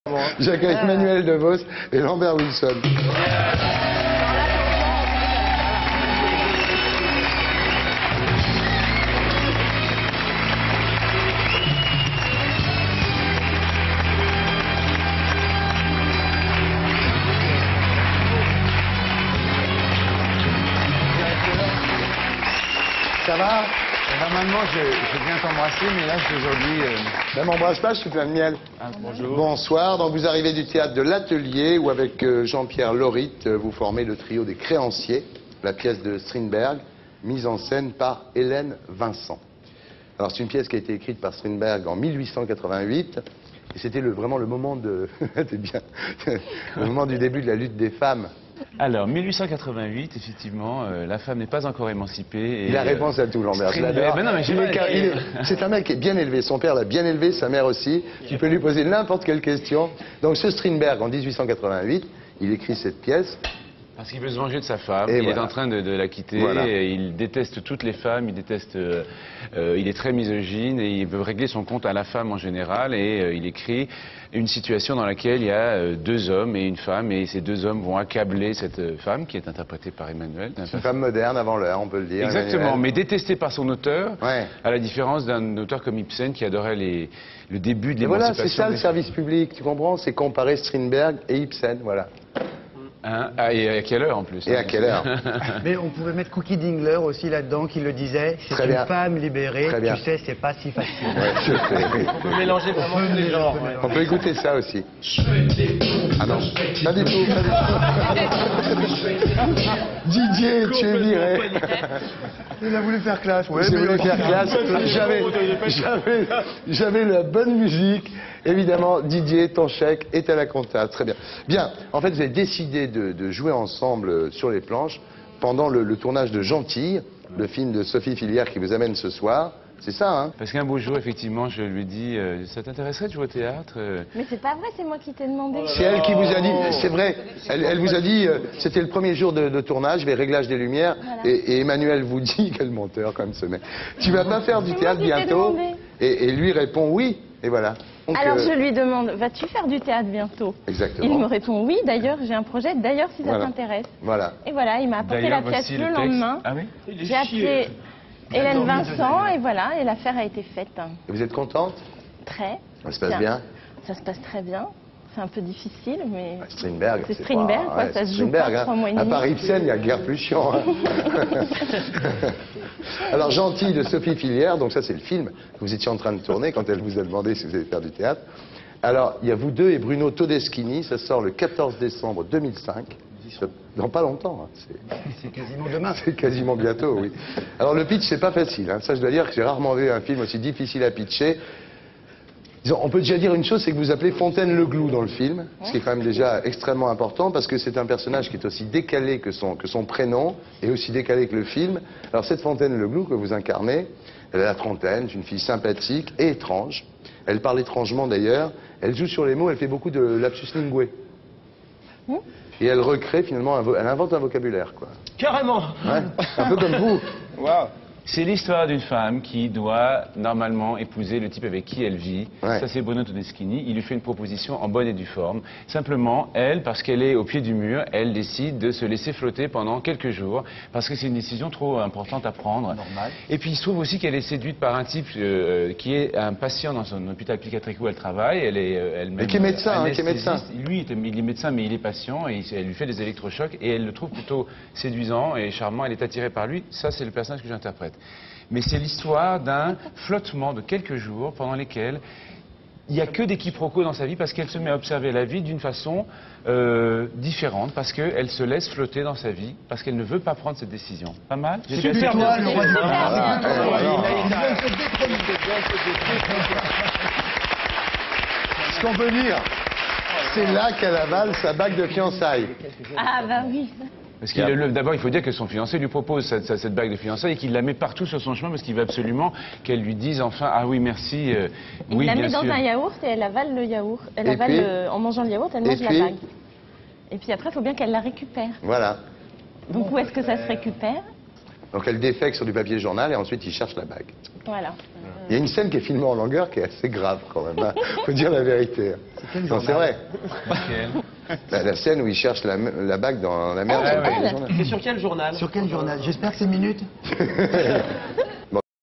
Interview actualité Emmanuelle Devos et Lambert Wilson - Archive INA
En présence de Samuel BENCHETRIT, lady LAISTEE, Stéphane ROUSSEAU, Eric et Ramzy, Thierry ARDISSON reçoit les acteurs Emmanuelle DEVOS et Lambert WILSON venus pour la pièce de théâtre "Les créanciers" et pour le film "Gentille". Images d'archive INA